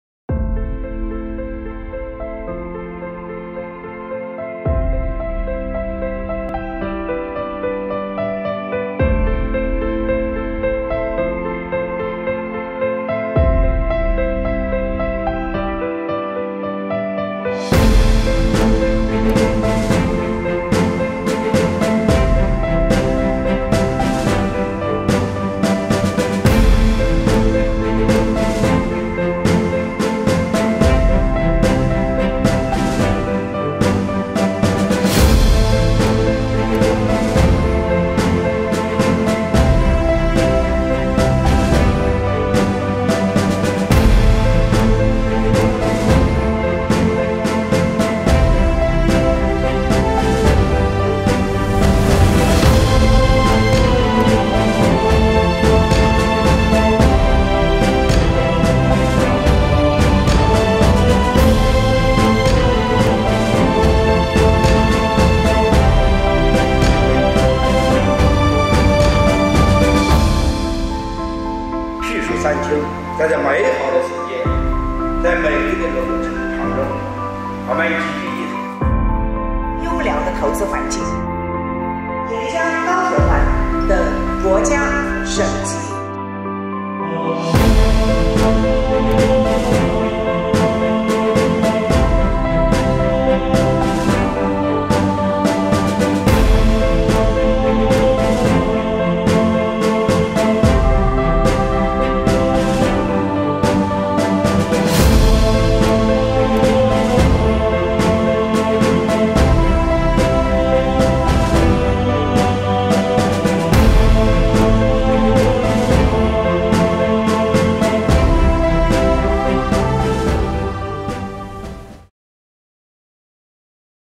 2020 Animated Film Venture Capital Summit
XRender has hosted the 17th China Changzhou International Animation Art Week "2020 Animated Film Venture Capital Summit", October 27th.
It is a customized feast exclusively for animated filmmakers, it has brought the industry-leading production teams together to put creativity into practice, intending to explore more possibilities for animated film production.
Standing at the door of the best era of animated films, XRender is willing to joint every animator and march forward bravely.
XRender,reliable rendering expert beside you.